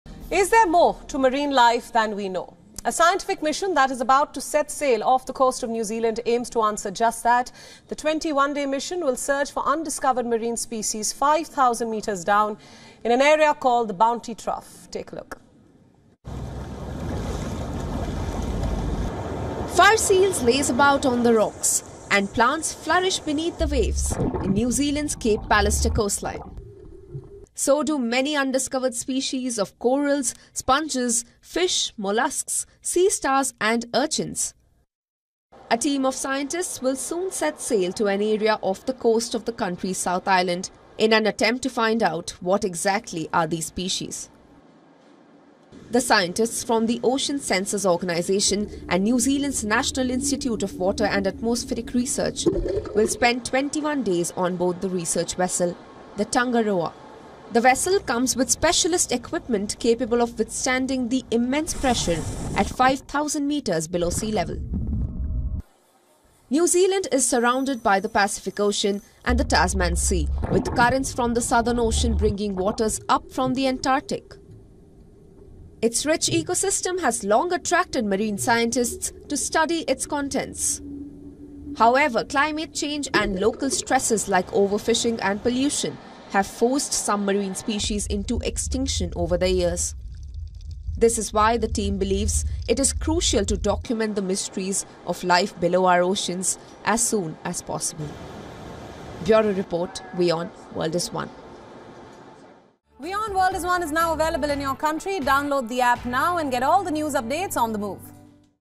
New Zealand's mission to search for undiscovered marine species | WION Climate Tracker
Is there more to marine life than we know? A scientific mission that is about to set sail off the coast of New Zealand aims to answer just that. The 21-day mission will search for undiscovered marine species 5000 metres down in an area called the bounty trough.